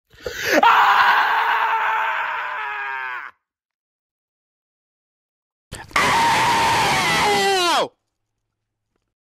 A Reupload of @ShaunTheYouTuber2001 Backup Channel & @UrLocalAustinDillonFan Screaming
Credit to them, but I still hate Shaun Sullivan, and I still have no thoughts on Jay Kizer.